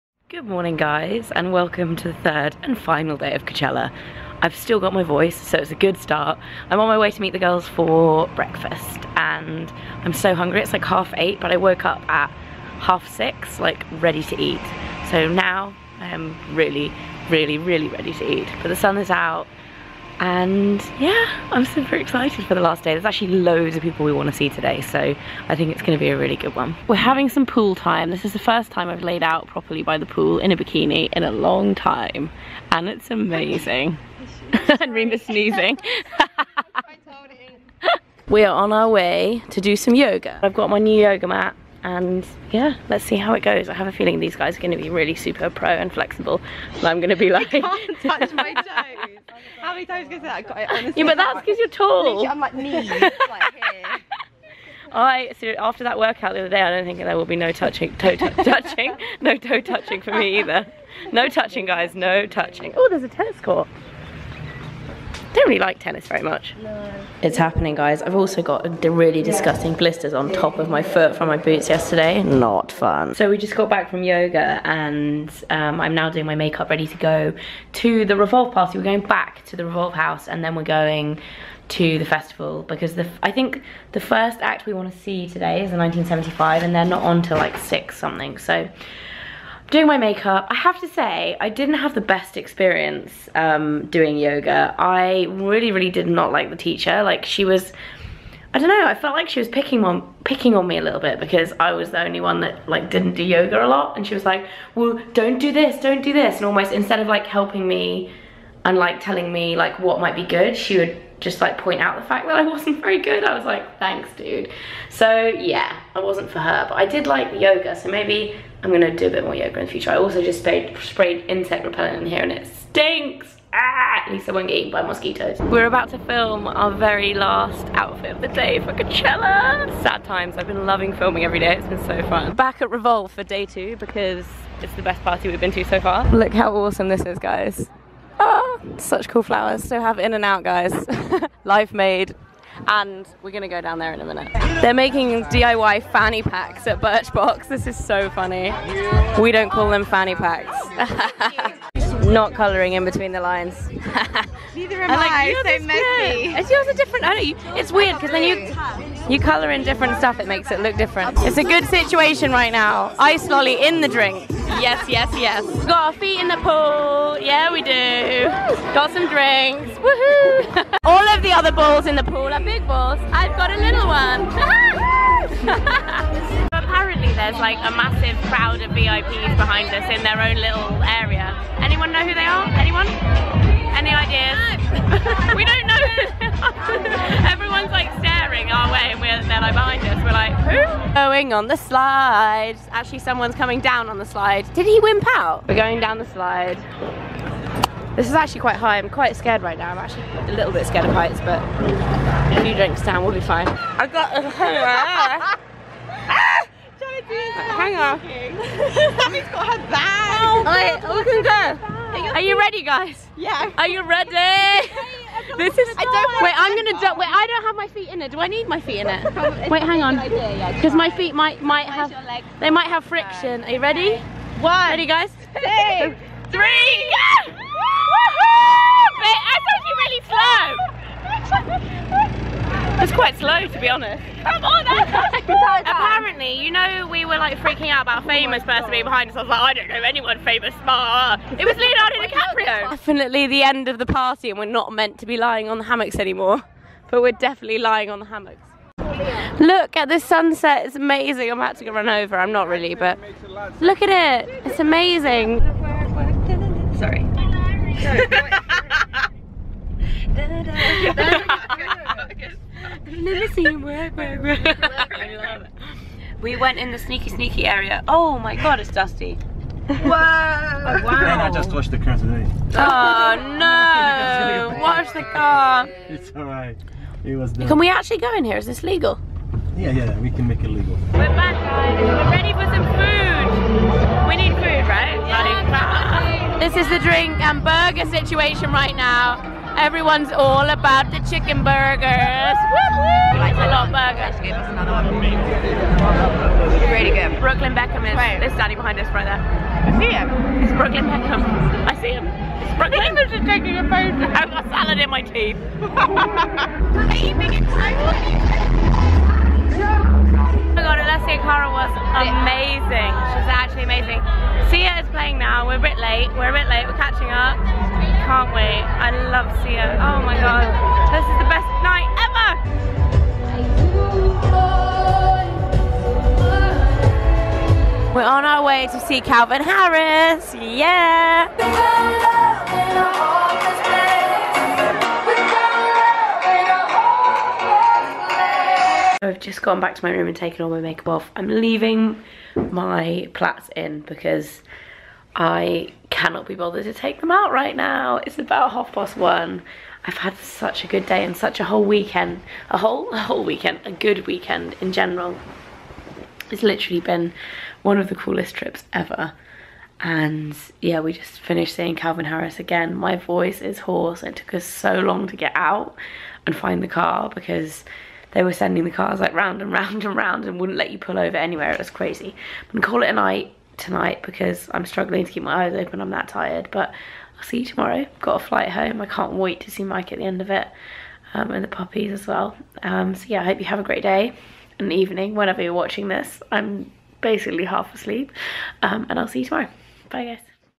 Pool PARTY & Tipsy Burgers! 17th April MfSpencer Michelle
Playlist The third and final day at COACHELLA! Heading to the Revolve pool party, crazy slides, tipsy burgers and watching Alessia Cara, Sia and Calvin Harris! EXPAND for more!